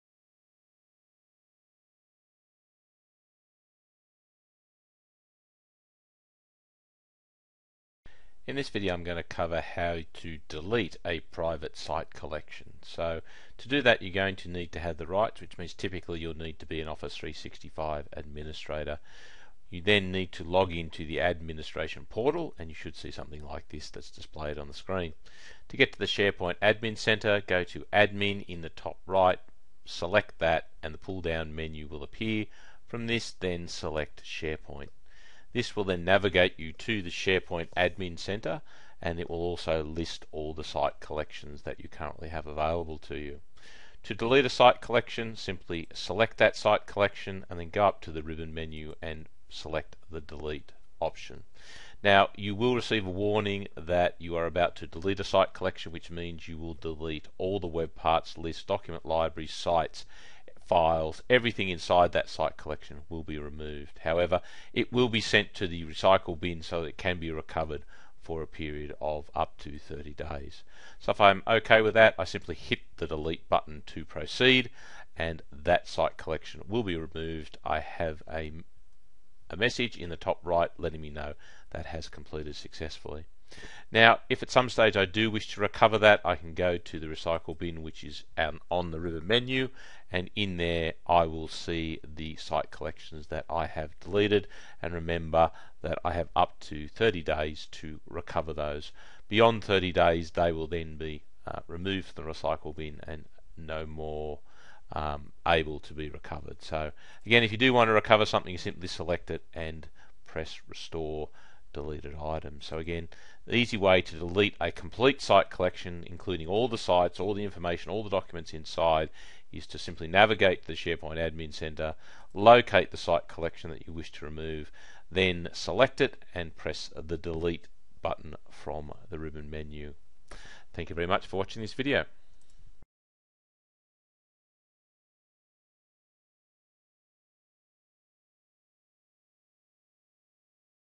Deleting a SharePoint Online Site Collection
This video will show you how to access the SharePoint Online admin center in Office 365 and delete an existing site collection. It will also demonstrate how deleted site collections can be recovered from the Site Collection Recycle Bin if desired.